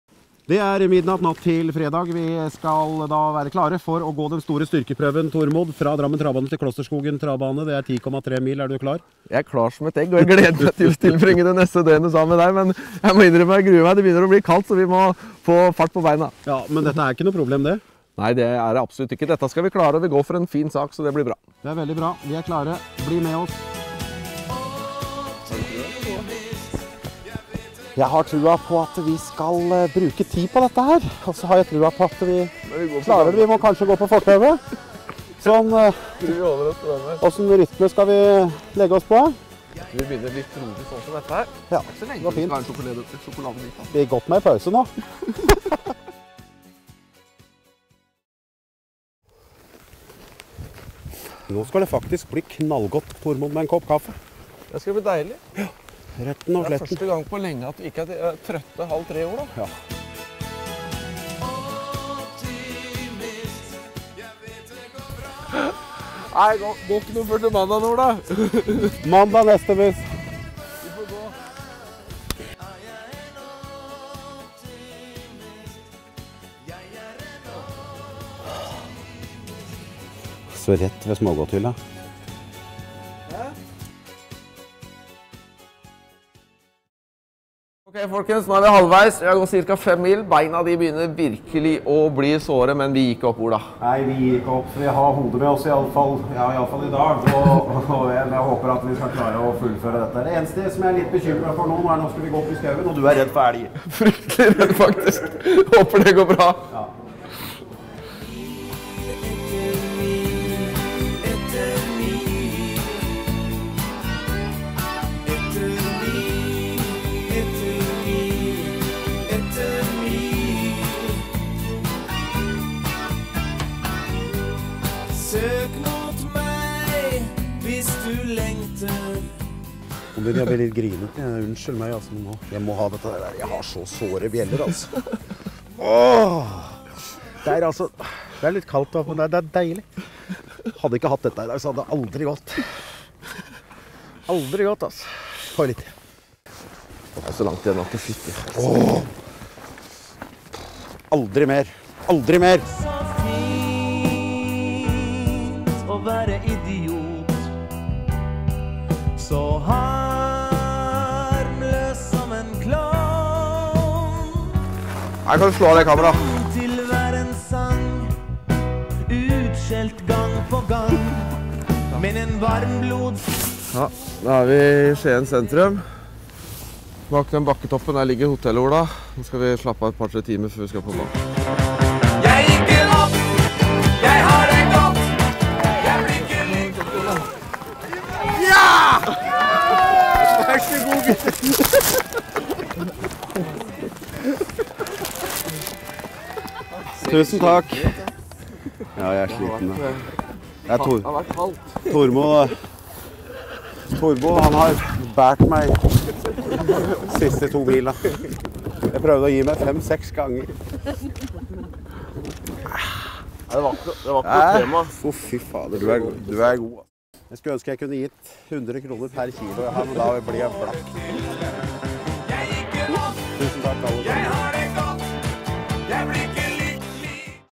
Tormod og Ola går til Klosterskogen
Se reisen til Tormod Hynne og Ola Slåtsveen fra Drammen travbane til Klosterskogen travbane i Skien.